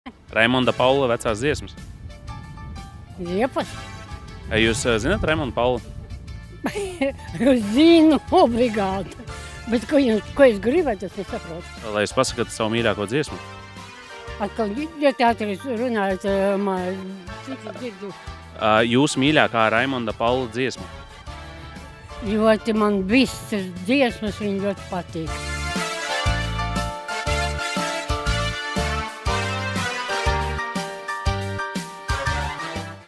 Vai Latvijas iedzīvotāji zin Maestro Raimondu Paulu un viņa mūziku?
🥳Maestro Raimonda Paula mūzikas cienītājiem "Latvijas mūzikas LIELKONCERTS" jau šo sestdien 10.septembrī Rīgā, Daugavas Stadionā.🥳